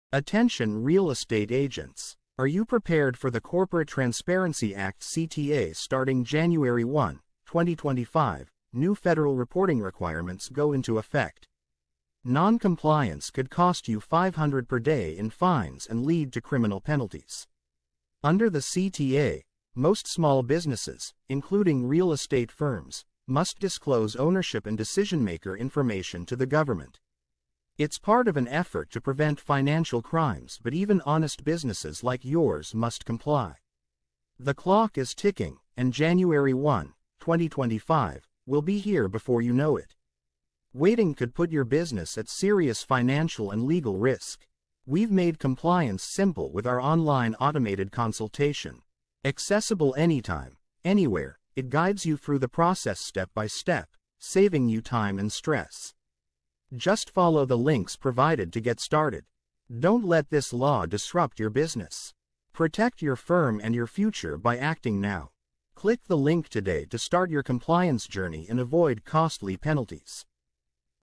Real Estate Agents: Don’t Miss the 2025 CTA Deadline!
Real estate agents, big changes are coming! The Corporate Transparency Act (CTA) takes effect on January 1, 2025, requiring new business reporting to avoid $500 daily fines and legal risks. In this video, we explain what the CTA means for your agency and how you can meet the requirements easily. Start now with our online automated consultation—click the link for more info!